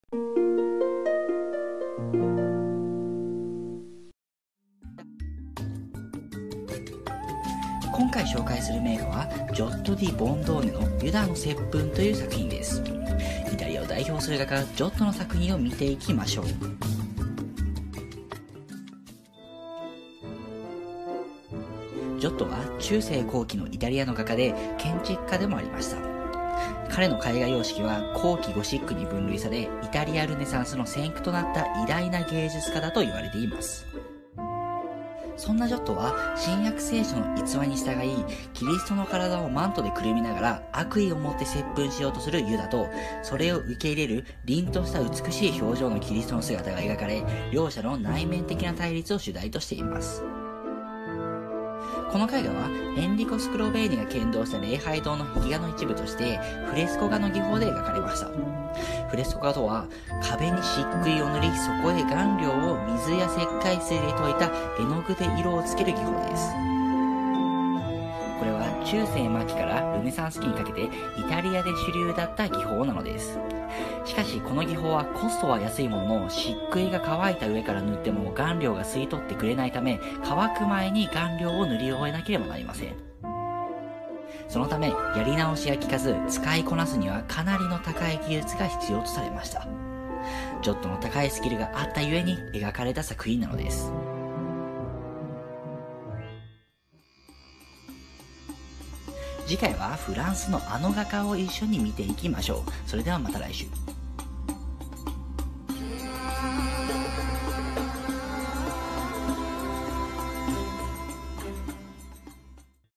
天才画家の技術の証明！？ジョット『ユダの接吻』を解説！
今回は、ジョット・ディ・ボンドーネの『ユダの接吻』を紹介しました。

解説・紹介してほしい絵画がありましたら、コメントしていただけると嬉しいです。

□instgram
tree_of_art_

□参考資料
・教養としての西洋美術史
・366日の西洋美術
・死ぬまでに観ておきたい世界の絵画1001

2分でちょっぴり知識人に。明日よりちょっと物知りに。
過去の美術家がどんな思いで、その絵を描いたのか。私と探りに行きませんか？

※知識に関しては徹底的に調べているつもりですが、専門家によって解釈の違い、表現の違いが存在しますのでご了承下さい。